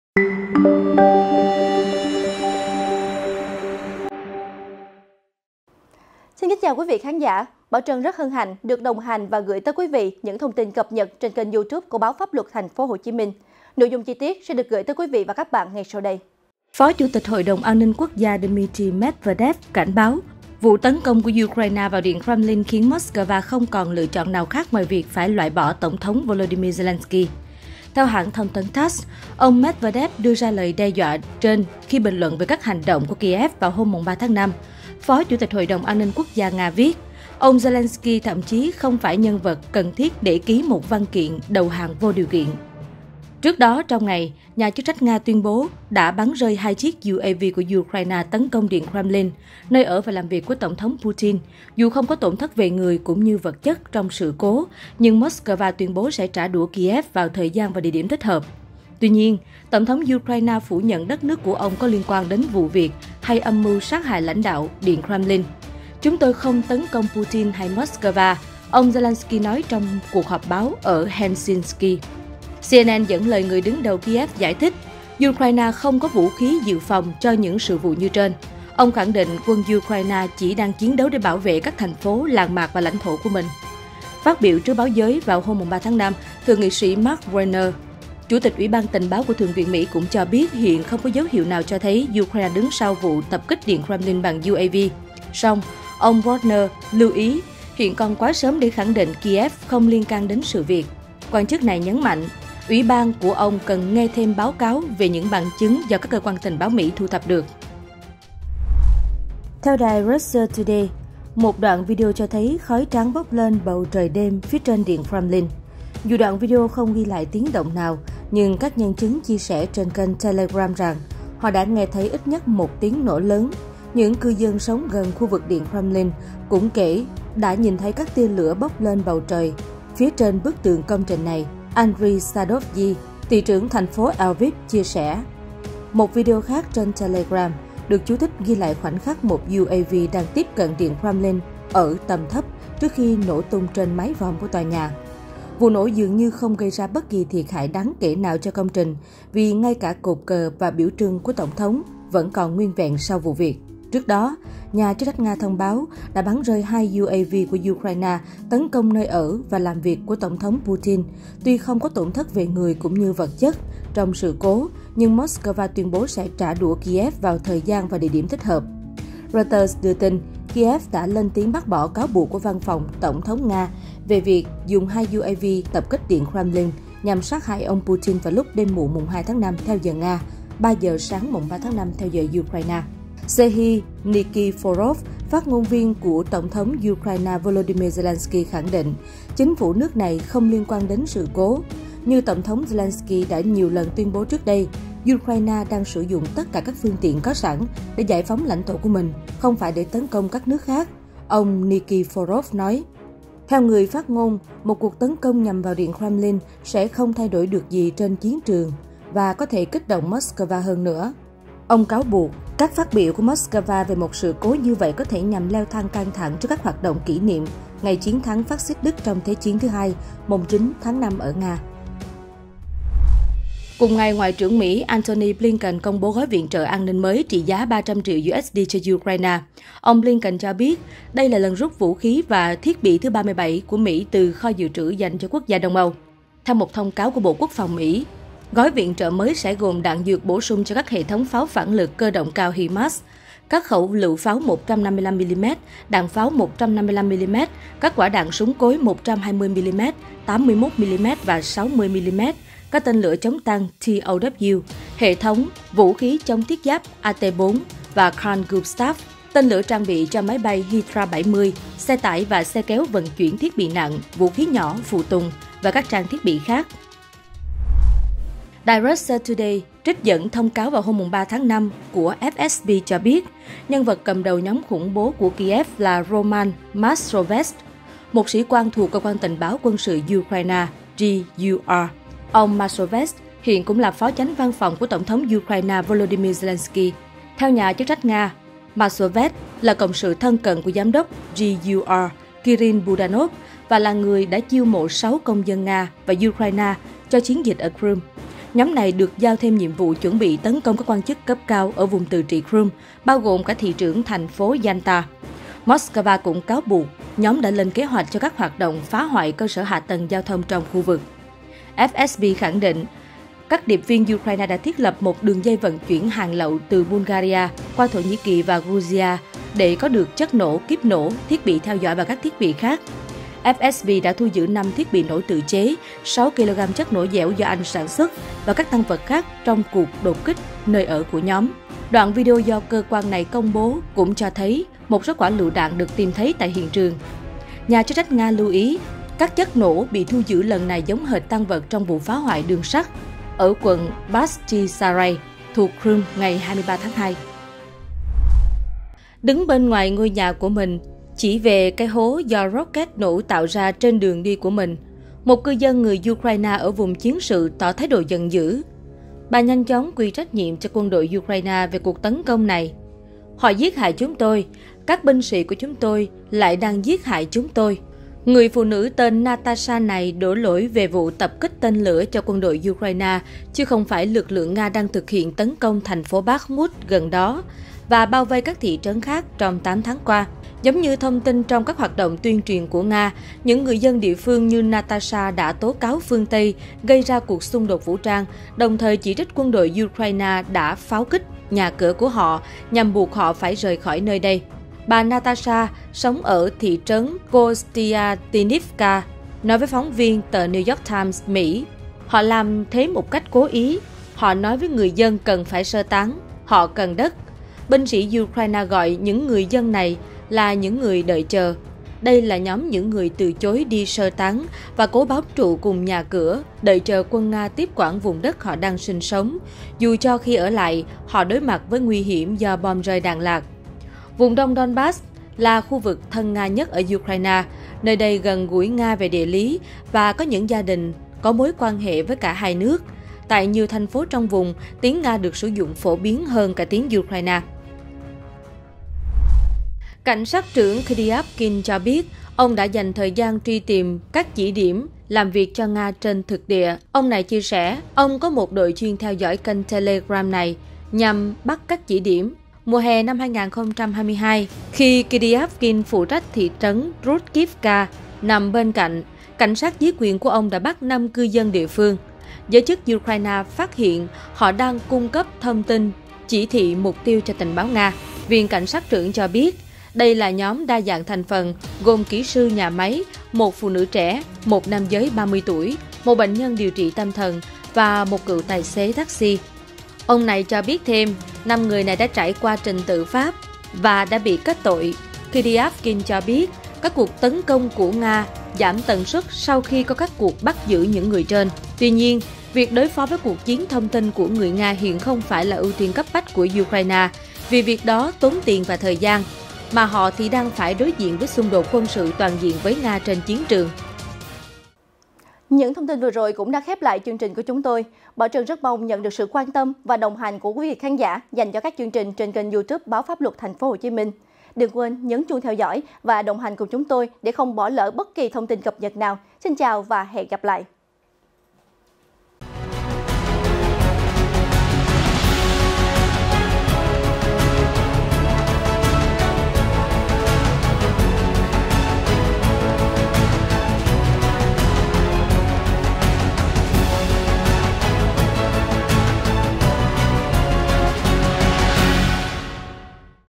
Sau vụ điện Kremlin bị tấn công, ông Medvedev kêu gọi “loại bỏ” tổng thống Ukraine | PLO
PLO | Phó Chủ tịch Hội đồng An ninh quốc gia Nga Dmitry Medvedev cảnh báo: vụ tấn công của Ukraine vào Điện Kremlin khiến Moscow không còn lựa chọn nào khác ngoài việc phải “loại bỏ” Tổng thống Volodymyr Zelensky.